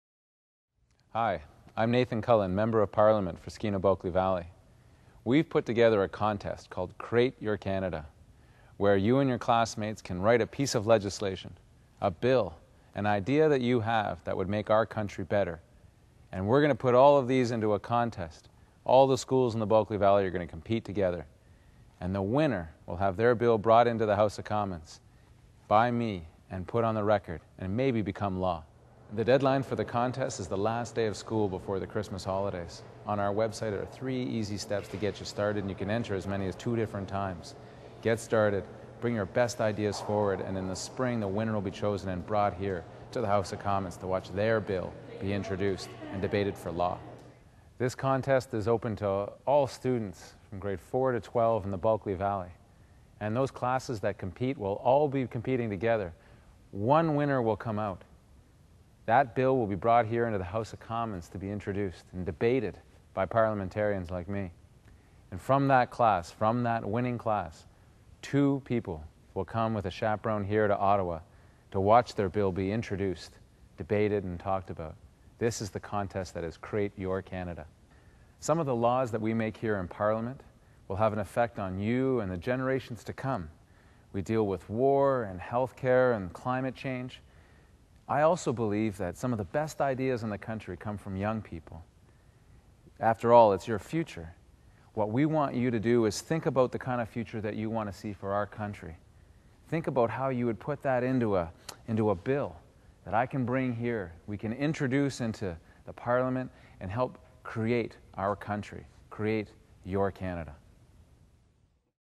Create Your Canada Contest
In this video, Nathan Cullen introduces the Create Your Canada contest.  In this contest, students in grade 5-12 think of ways to improve Canada, conduct research, and write bill proposals.  The students who create the winning bills will each earn a free trip to Ottawa to watch Nathan Cullen introduce their bill in the House of Commons.